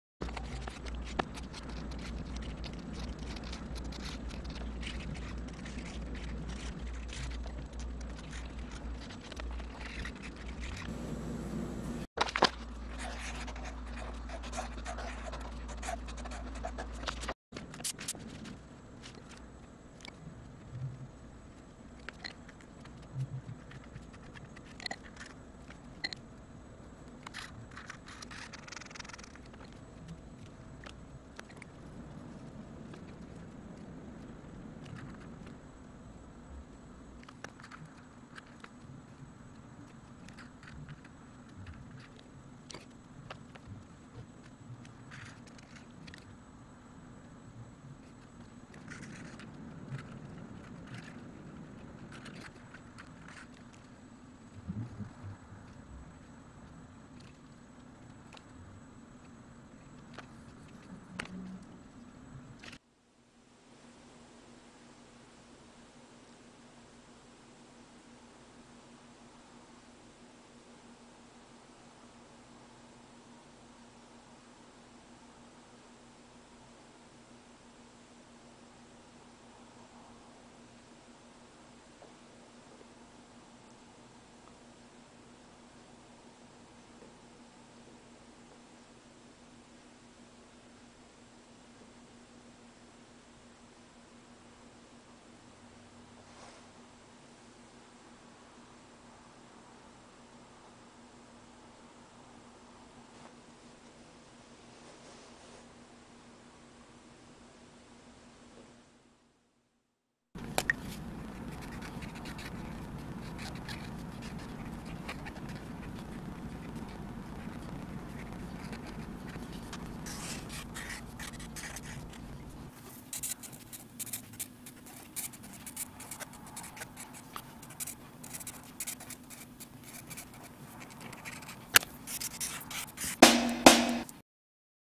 DAILY MONSTER 129
--- Timelapse video of Mr. Stefan G. Bucher creating a brand new monster every week.